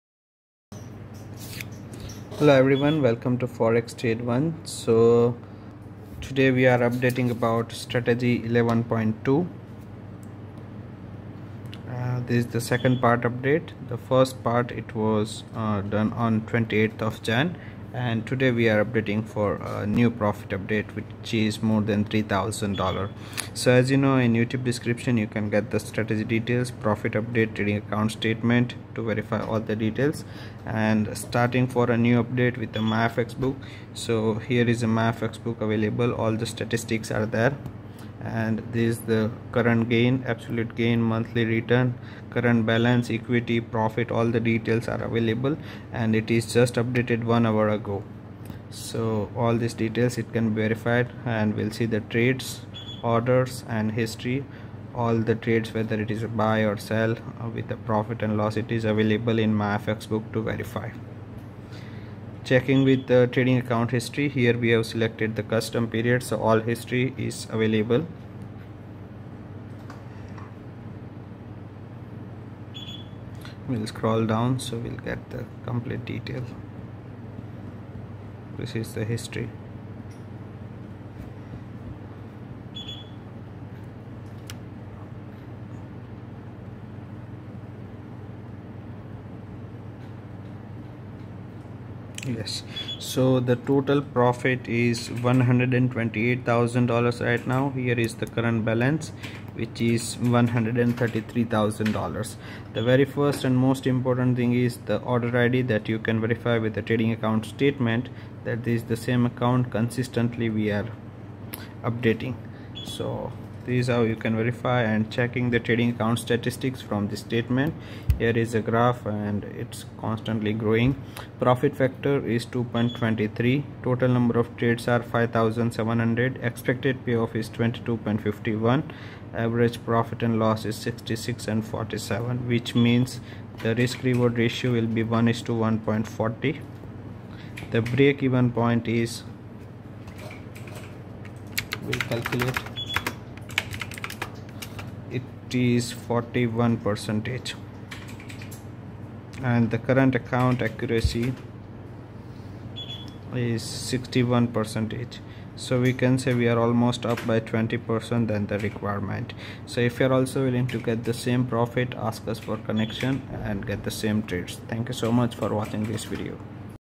STRATEGY 11.2 OCTA HEDGE PAIR 1283% TOTAL ROI & 33% 5 DAYS ROI PART 2
00:00 Introduction
00:30 MyFxBook Portfolio
01:00 How does strategy work
01:55 Trading Statement Analysis